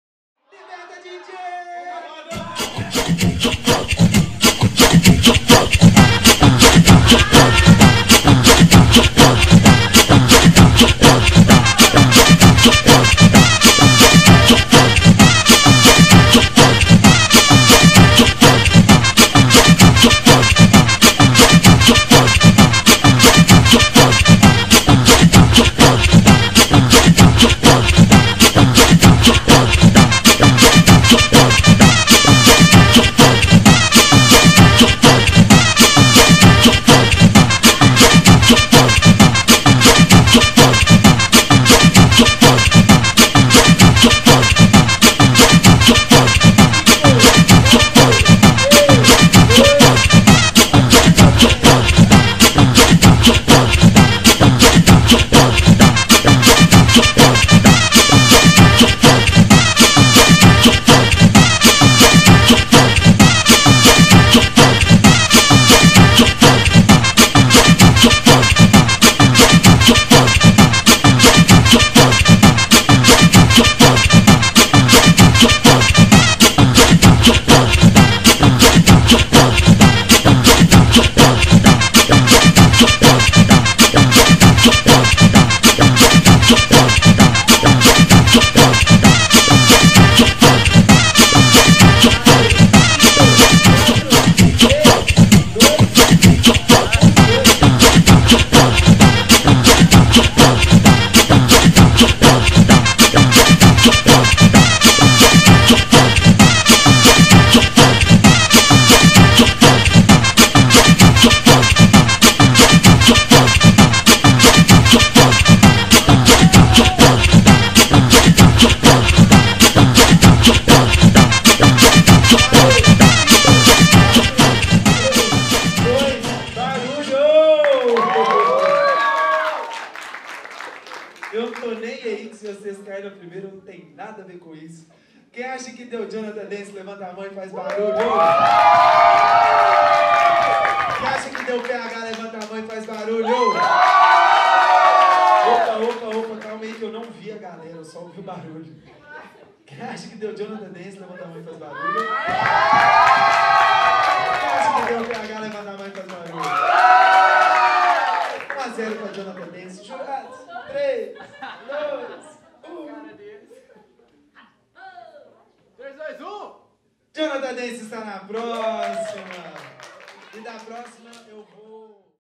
Disputa Nervosa - PH Dancy vs Johnatam Dancy (4ª Final) 10/06/2016
Edição da Disputa Nervosa aconteceu dentro da programação da 3ª Mostra Benjamim de Oliveira, no Tambor Mineiro/Belo Horizonte.